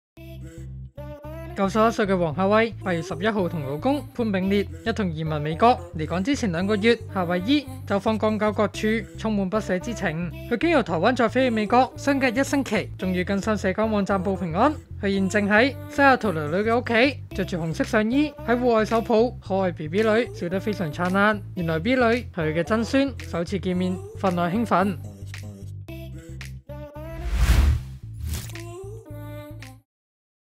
黃夏蕙移民抵西雅圖報平安 首見曾孫勁興奮︰陪住個塞女去遊花園
91歲的黃夏蕙，8月11日與老公潘炳烈一同移民美國，離港前兩個月夏蕙姨走訪港九各處，充滿不捨之情。她經由台灣再飛美國，相隔一星期終更新社交網報平安。